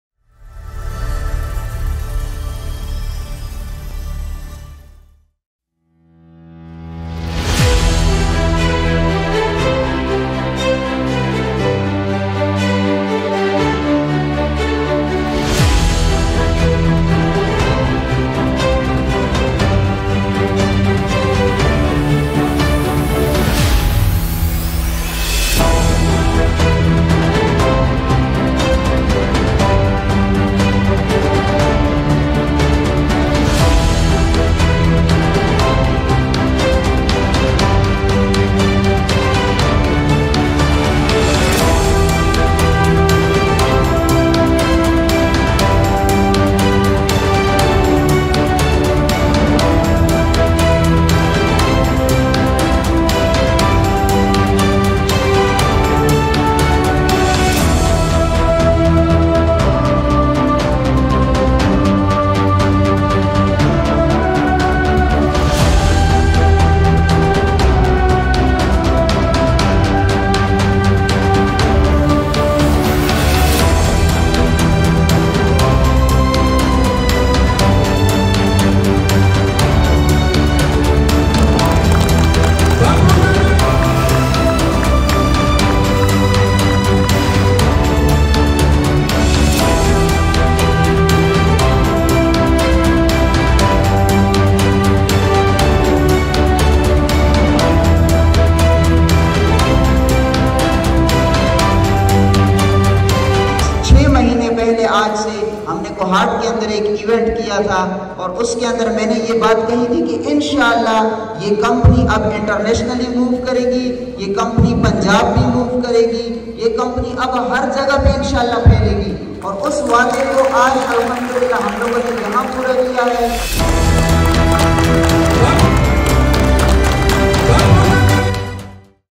As Group Of Companies Lunched Regional Office Lahore | Sanjo Tv Kohat
Breaking News, Sports, Entertainment and Religious at one place Only On SANJO TV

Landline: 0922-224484